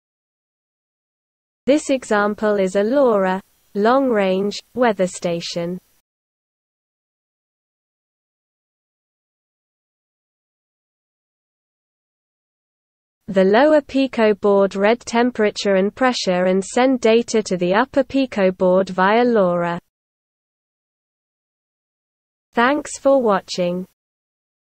Raspberry Pi Pico LoRa weather station | Arduino IDE
* LoRa module
* BMP180 temperature and pressure sensor

IDE : Arduino IDE
Language : C